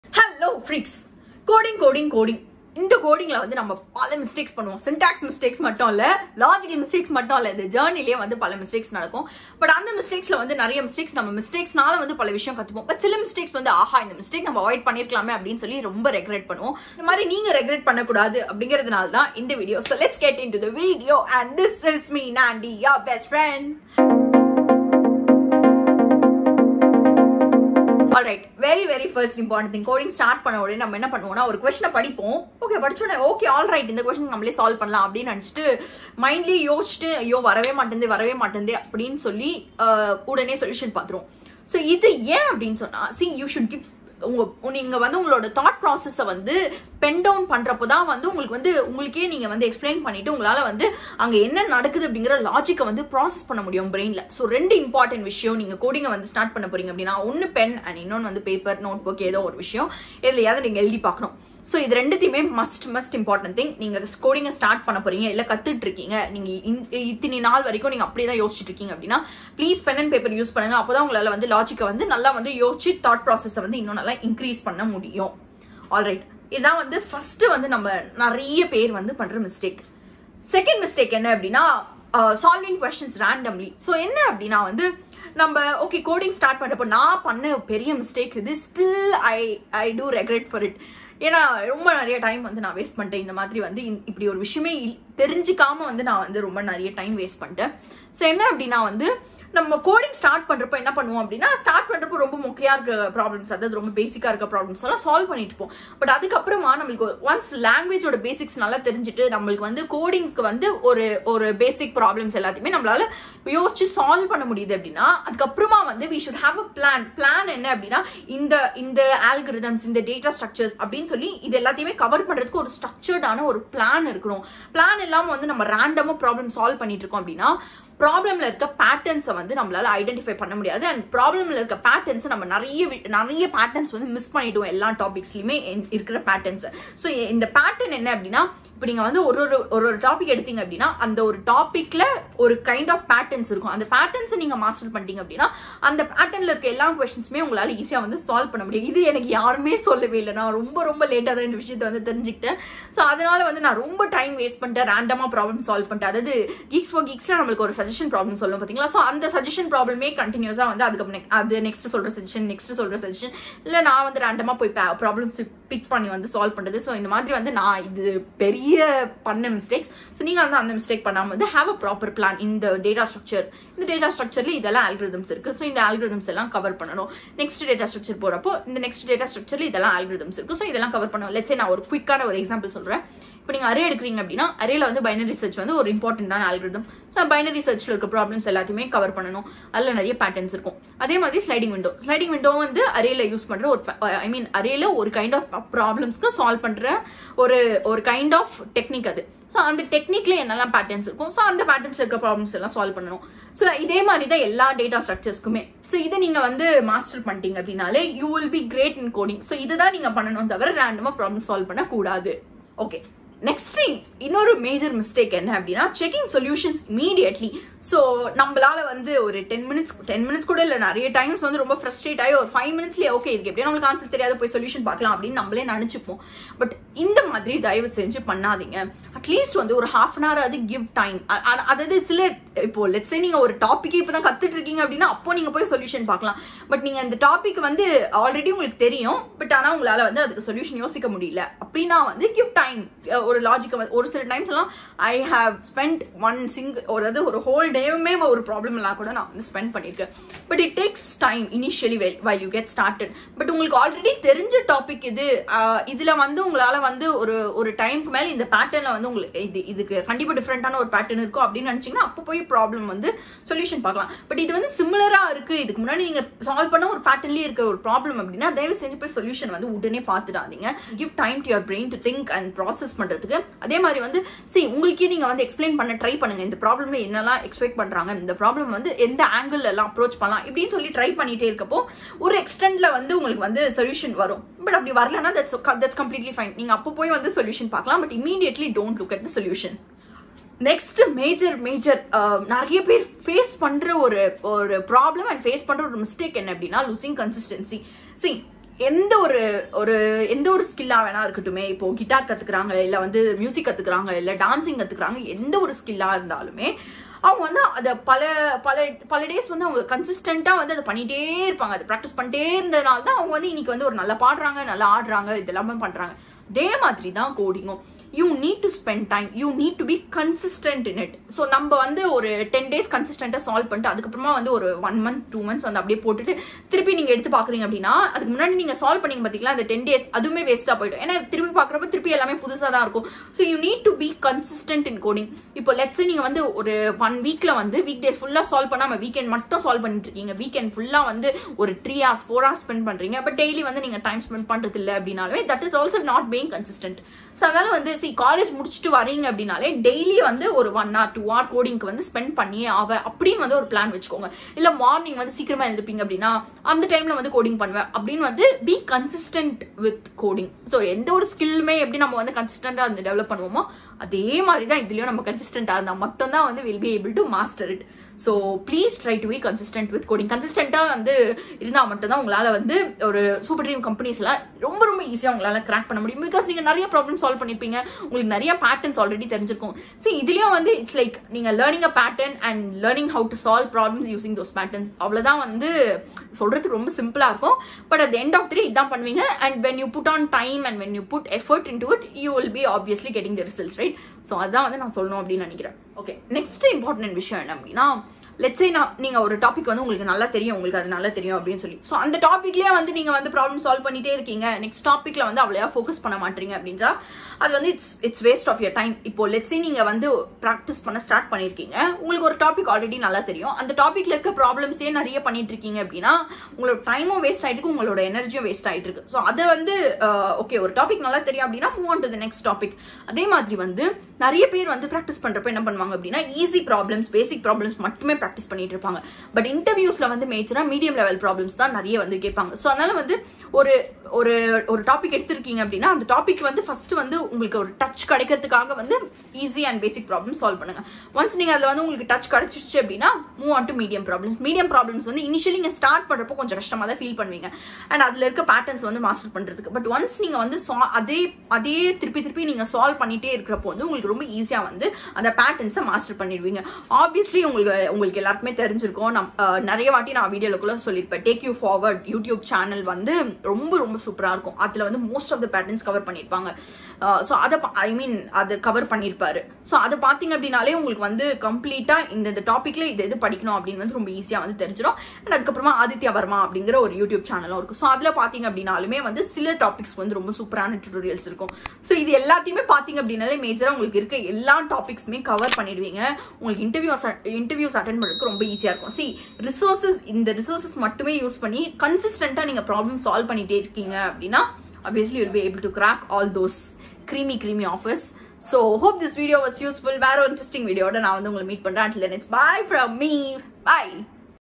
5 Coding Mistakes I wish I had avoided in College(Tamil)😭🛑I missed better package becoz of this
Generally, we tend to make lot of mistakes while learning to code like Syntax errors, logical errors and so on. But we are not going to talk those. In this video, we wanted to let you know the mistakes that you make in the process of learning to code.